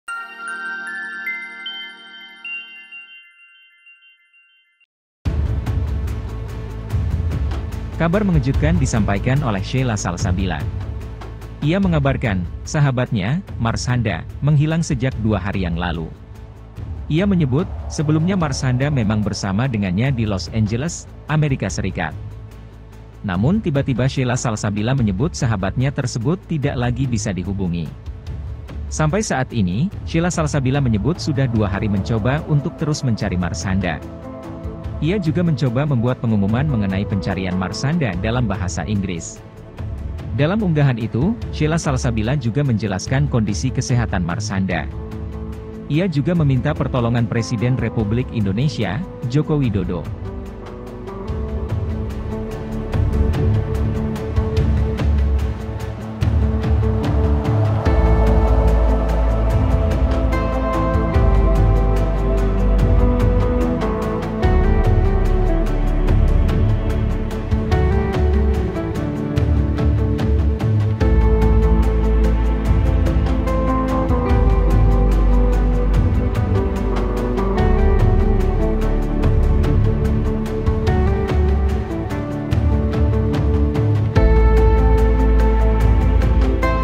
Marshanda Dikabarkan Hilang di Amerika
Kabar mengejutkan disampaikan oleh Sheila Salsabila. Ia mengabarkan, sahabatnya, Marshanda, menghilang sejak dua hari yang lalu.

Ia menyebut, sebelumnya Marshanda memang bersama dengannya di Los Angeles, Amerika Serikat.

Namun tiba-tiba Sheila Salsabila menyebut sahabatnya tersebut tidak lagi bisa dihubungi.

Sampai saat ini, Sheila Salsabila menyebut sudah dua hari mencoba untuk terus mencari Marshanda.